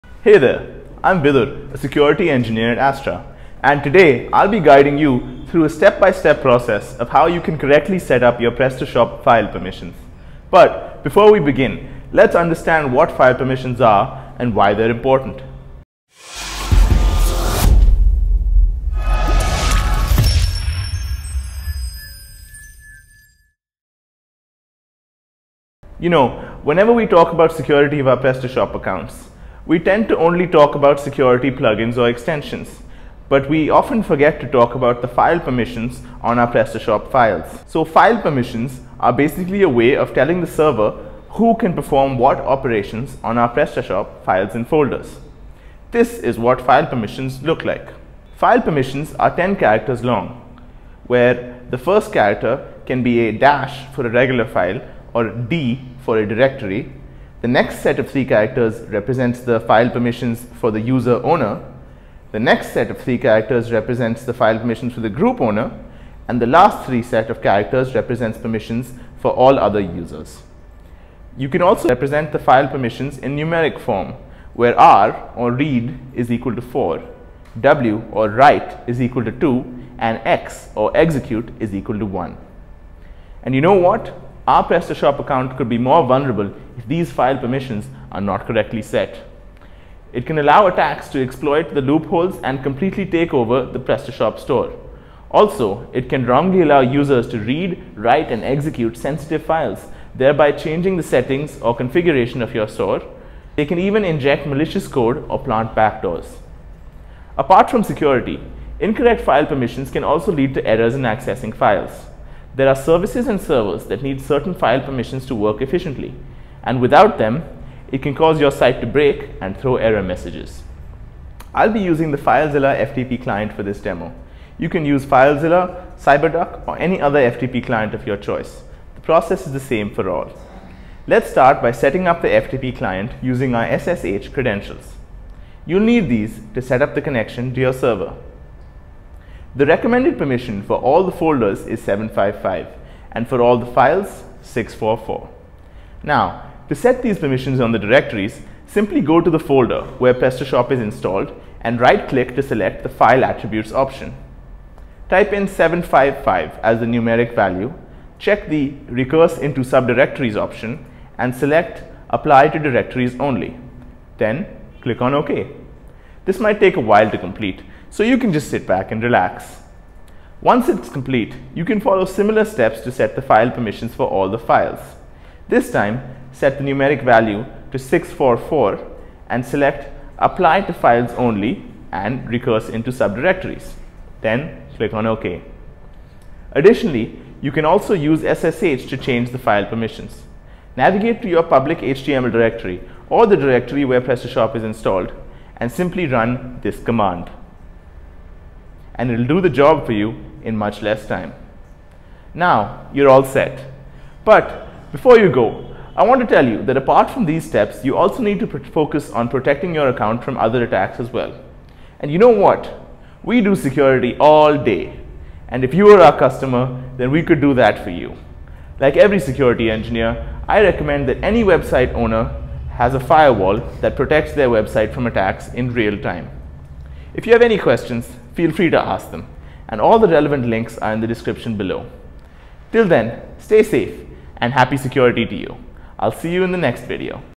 PrestaShop File & Folder Permissions | Setup PrestaShop File Permissions Via FileZilla (FTP & SSH)
Let's understand what are Prestashop File Permissions and why are they important.

File permission is basically a way of telling the server that who can perform what operations on our PrestaShop files & folders

This is how file permission would look like:
-rwxrw-r--
drwxrw-r--

You can assign users three type of permissions
r - read  - 4
w - write - 2
x - execute - 1

And you know what, our PrestaShop accounts can be more vulnerable if these file permissions are not correctly set.

It can allow attacks to exploit the loopholes and completely takeover the PrestaShop store. Also, it can wrongly allow users to read, write and execute sensitive files thereby changing the setting/configurations of your website or can also inject malicious code or plant a backdoor.

Using FileZilla

Let's start by setting up the FTP client using ssh credentials

You'll need these to setup the connection to your server:
Host - sftp://server_ip or sftp://
Port - 22
username
password

The recommended permission for all the folders is 755 and for all the files, it’s 644.

Prestashop Folder/File Permissions Via SSH

Additionally, you can also use the SSH to change the file permissions. Navigate to your public_html directory or the directory where Prestashop is installed. And Simply, run this command “find . -type f -exec chmod 644 {} \; && find . -type d -exec chmod 755 {} \;” and it’ll do the job for you in much lesser time.